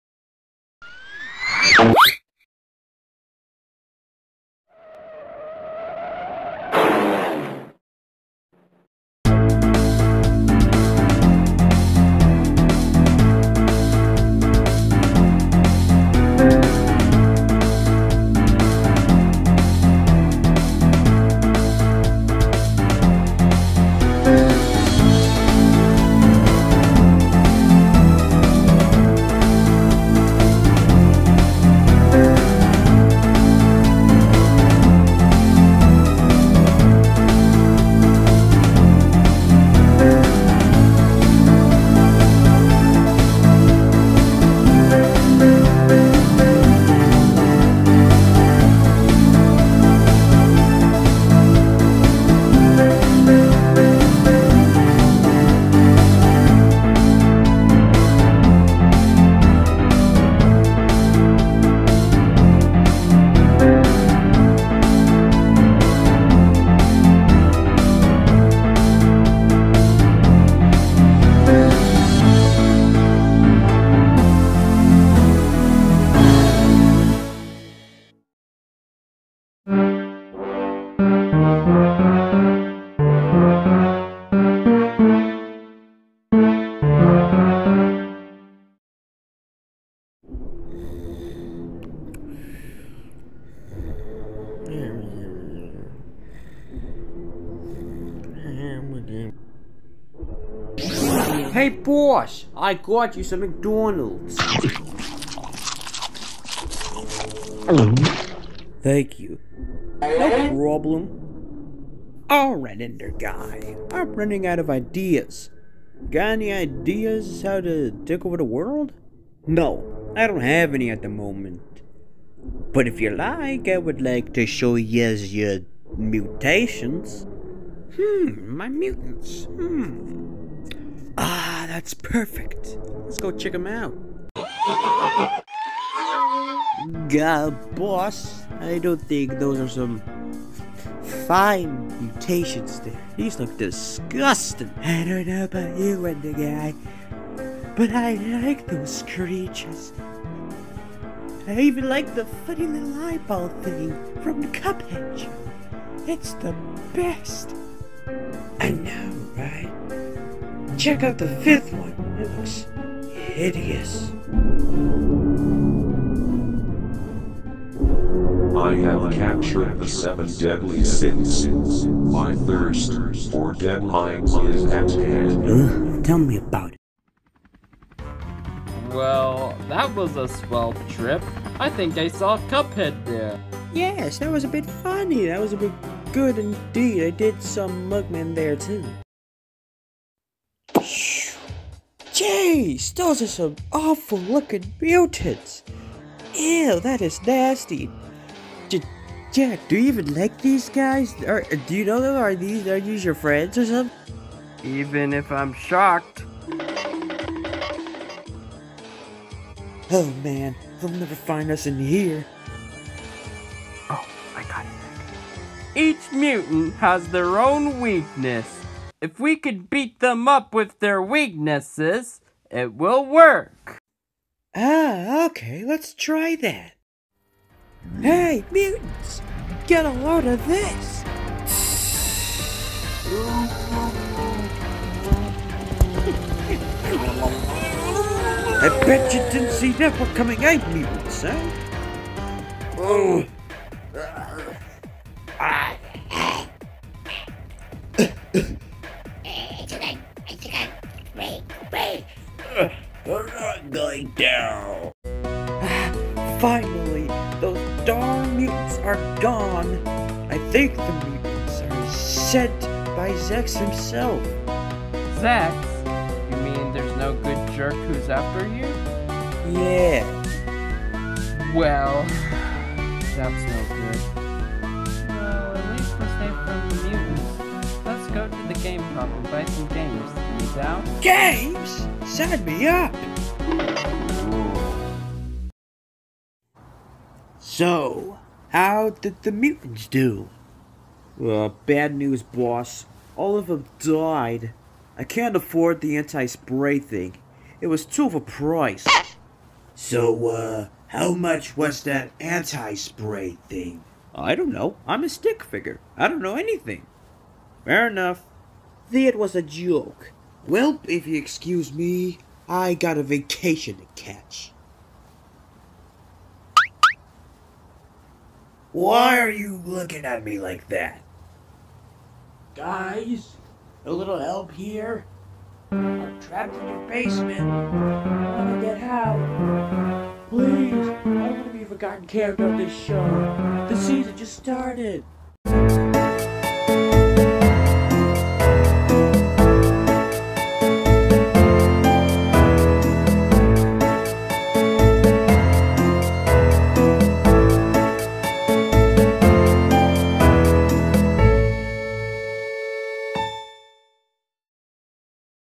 The vico bruh show! Episode 6 - Zex's Finest Mutations
the first Vico bruh episode of 2023! whoopee!

In an attempt to hunt Vico and Company down, Zex decided to Summon his Finest mutants! and they are.. something I'll tell you that..

Voice talent of yellow star is done by ‎@jackebicdude536